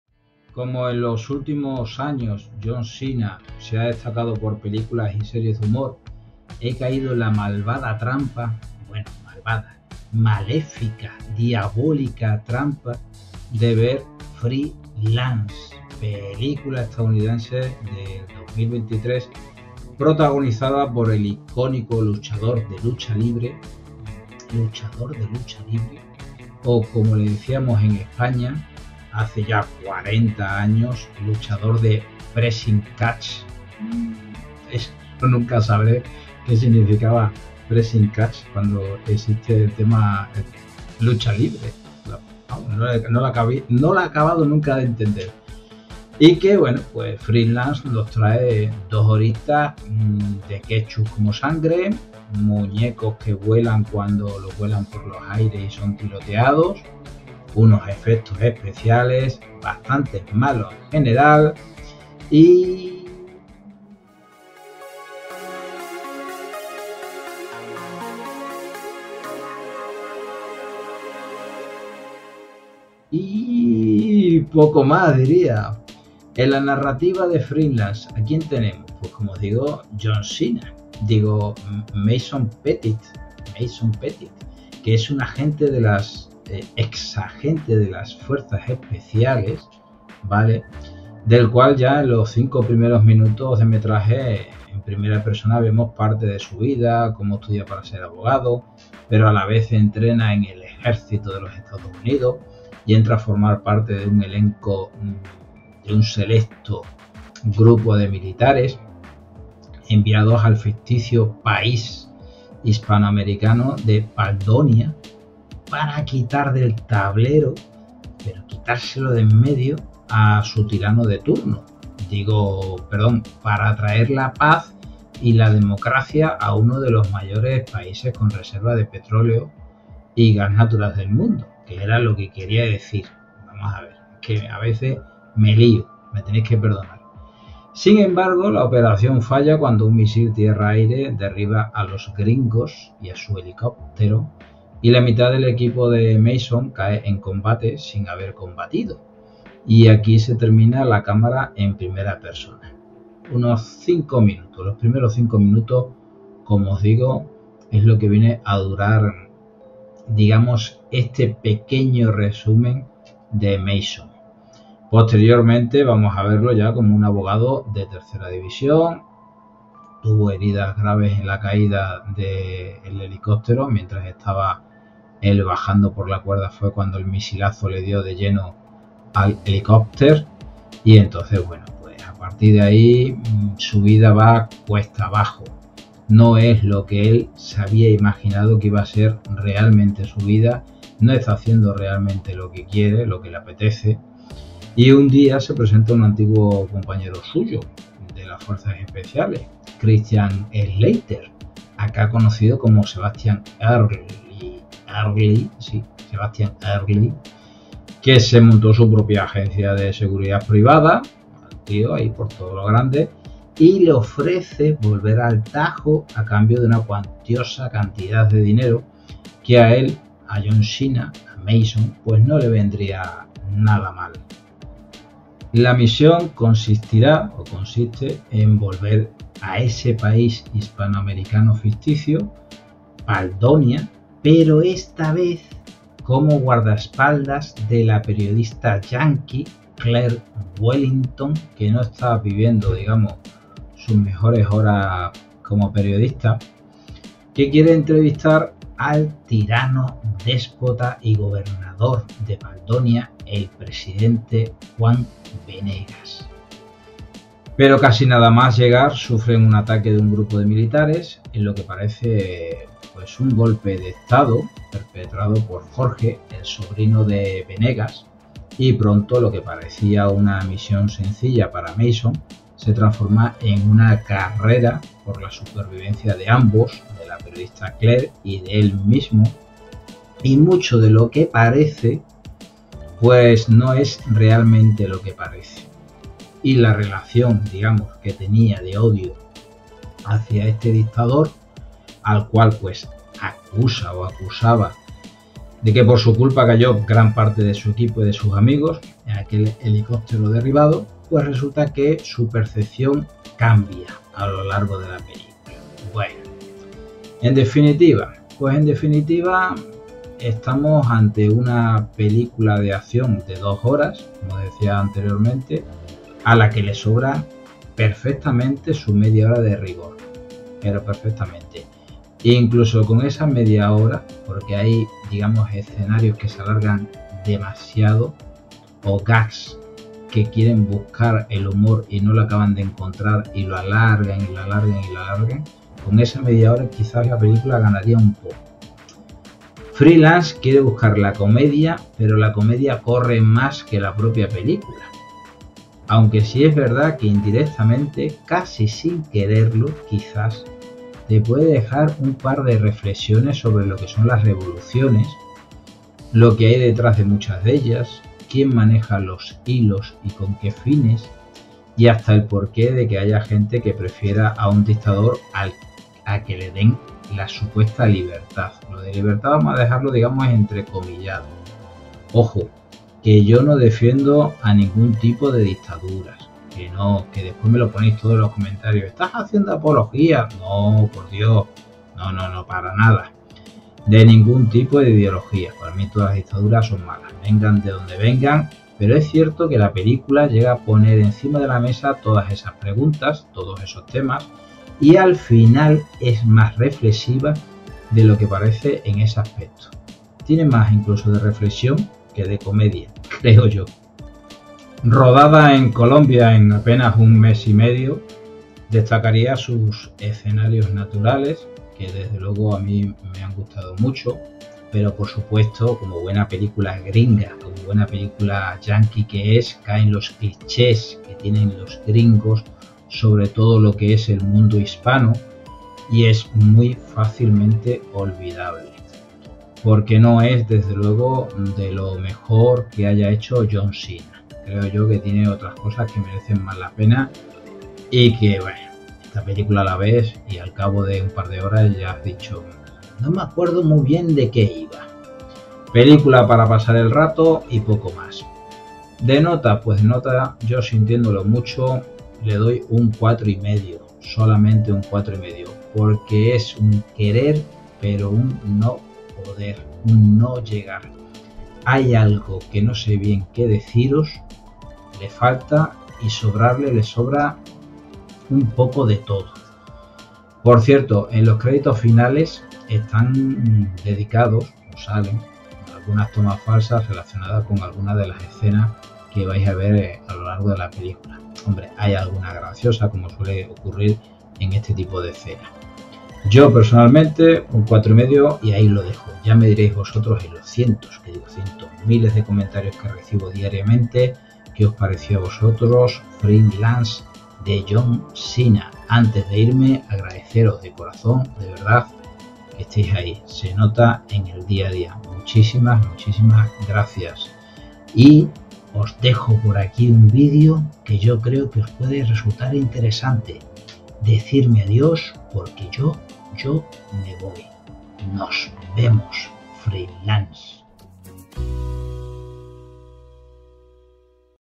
Freelance (2023)
Como en los últimos años, John Cena se ha destacado por películas y series de humor, he caído en la malvada trampa de ver “Freelance”, película estadounidense de 2023, protagonizada por el icónico luchador de la lucha libre, o como le llamábamos en España hace cuarenta años, Presing Catch, que nos trae ketchup como sangre, muñecos que vuelan o mueren tiroteados y unos efectos especiales bastante malos en general…
       ¿Por qué, dónde, cuando y cómo verla? Todas esas dudas no serán resueltas en este vídeo, pero queda estupendo ponerlo por aquí... Total, nadie lee las descripciones... ¿O sí?

Marcas de Tiempo:

00:00 Una Intro a lo John Cena...
01:14 Nos vamos a Paldonia...
08:12 Suscríbete, páaajaaroh